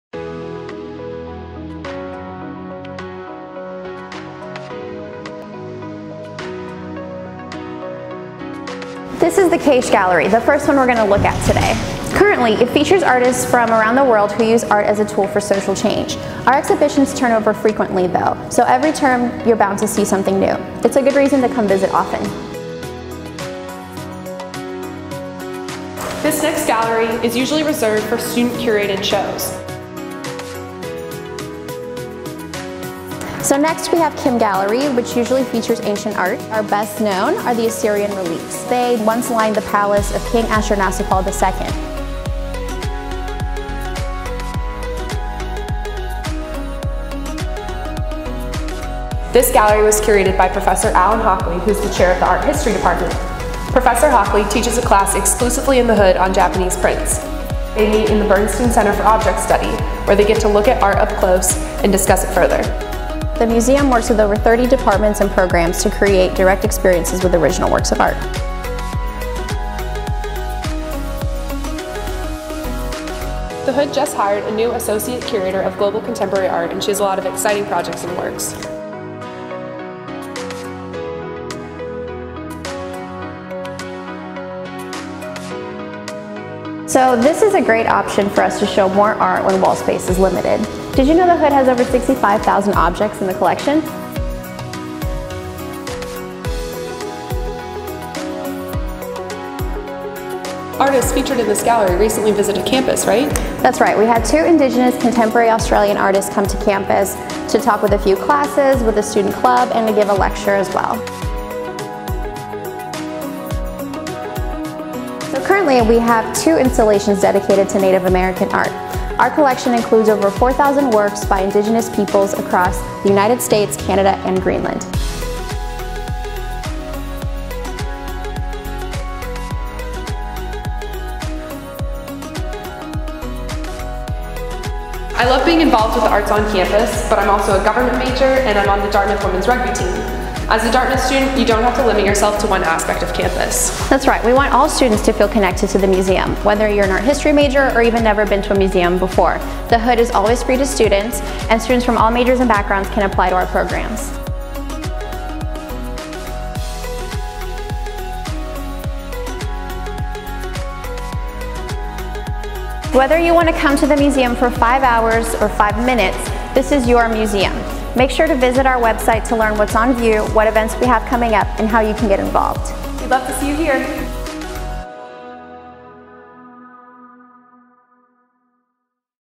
Tour the Hood Museum!
Campus Engagement Coordinator Isadora Italia and student intern Jules Wheaton '19 offer an inside look at the newly renovated Hood Museum of Art.